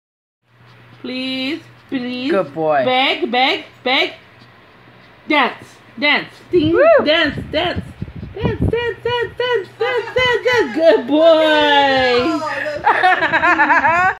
Excited dog breaks into happy dance for food
By the looks of it, Simba just can't hold back her dance moves when it comes to getting her paws on a treat. That's one impressive twirl!